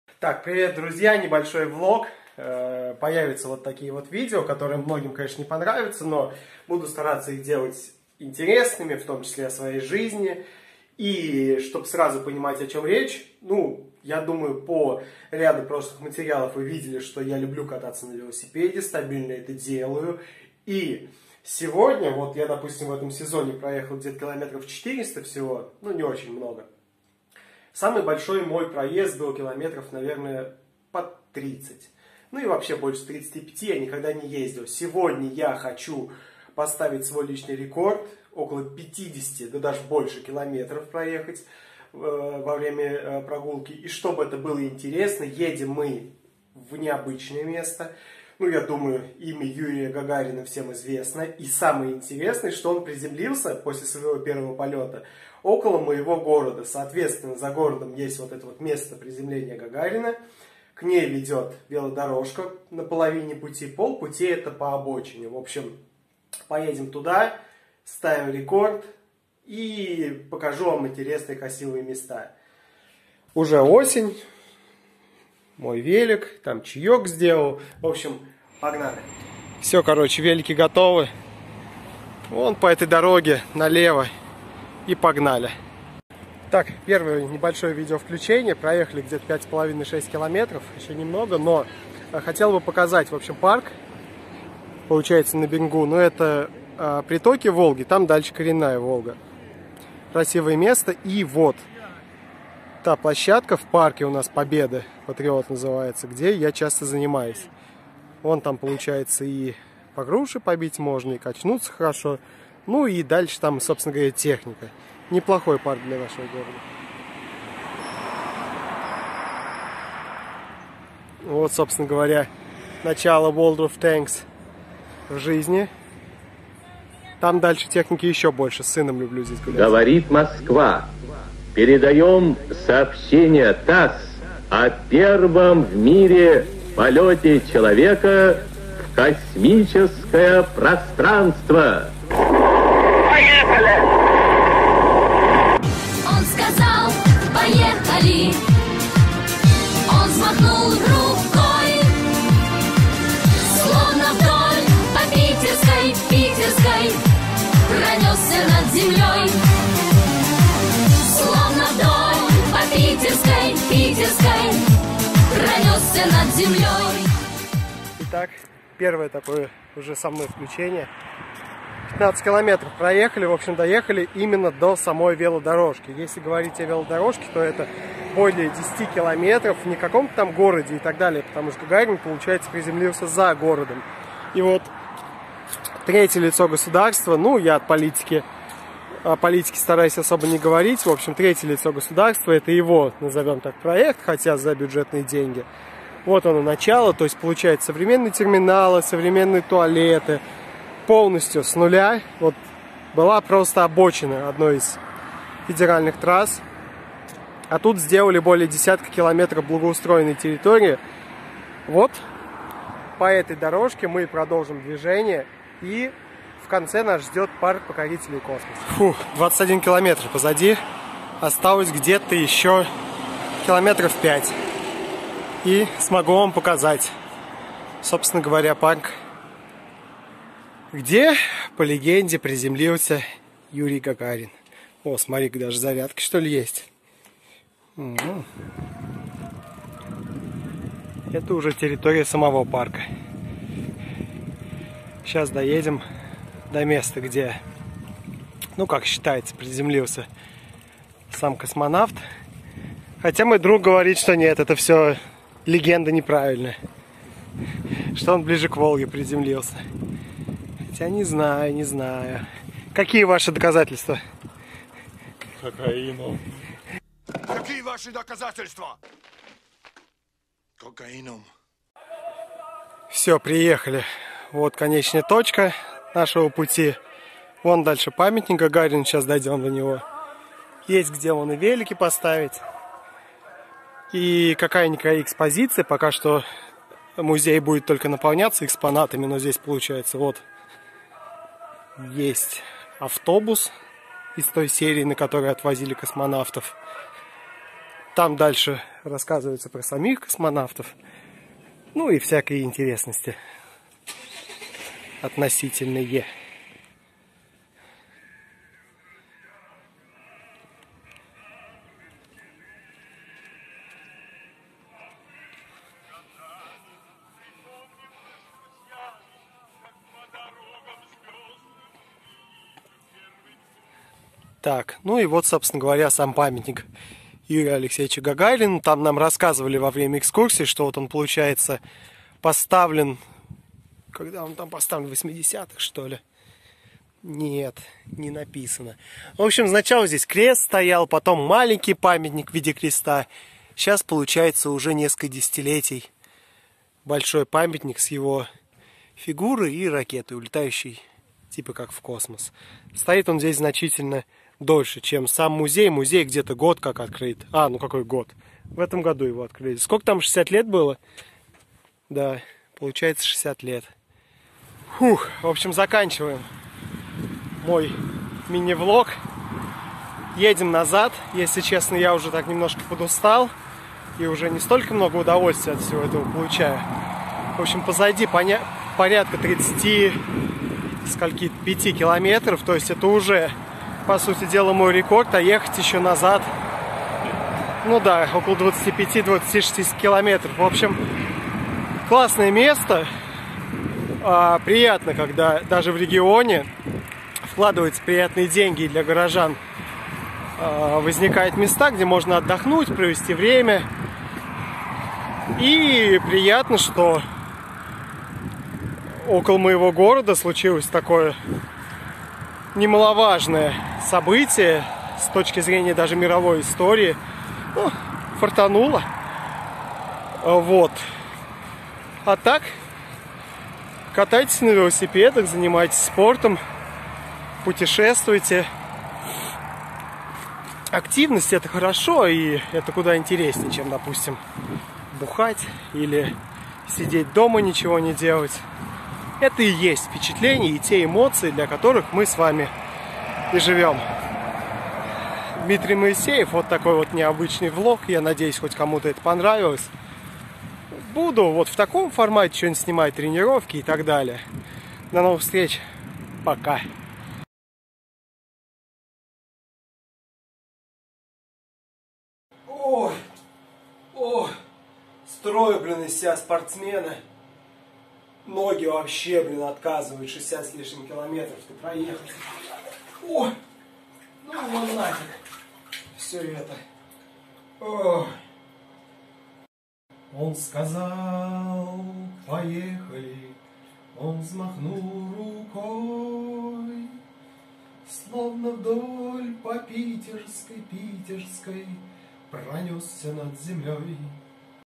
62 км на велосипеде к месту приземления Гагарина
Велопробег из города Энгельс к месту приземления Юрия Гагарина - общий пробег более 60 километров.